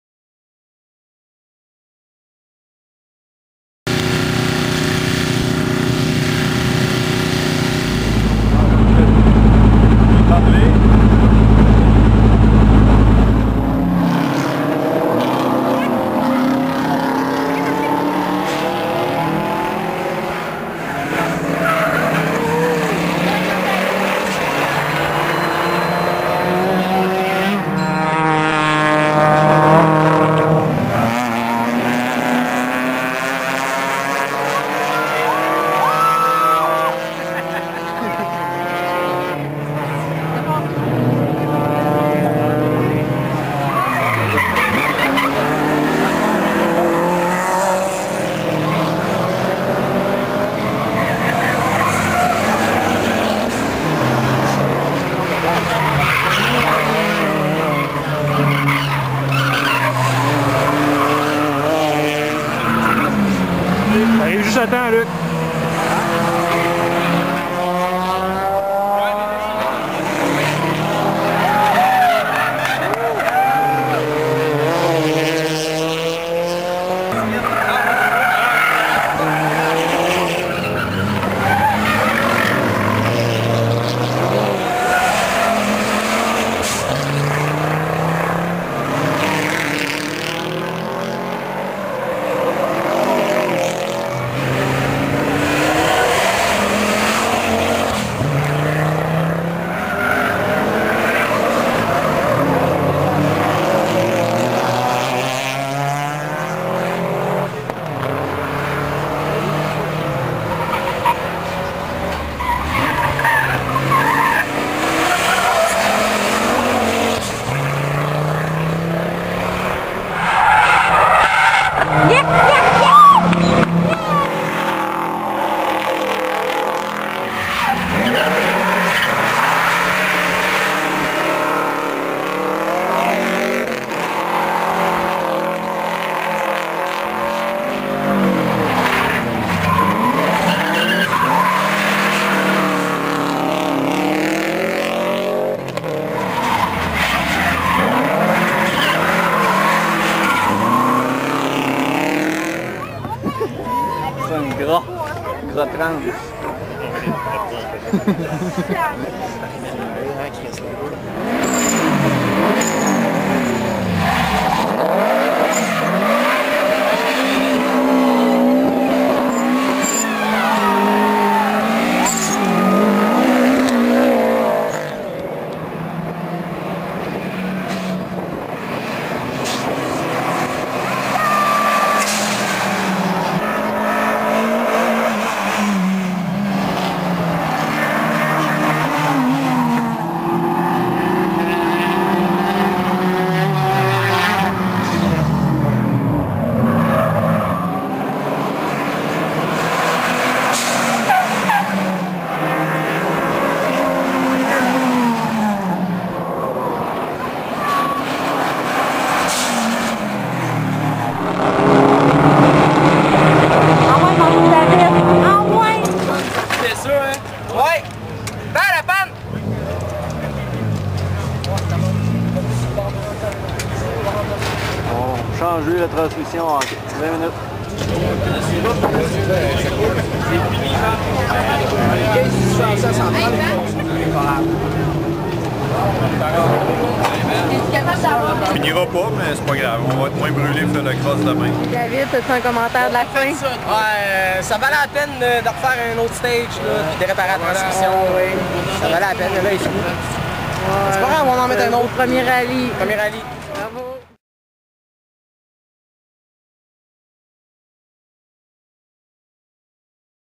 Rallye Sanair 2008 Platto
Championnat 4RM CASLL et coupe du québec.  Équipe Platto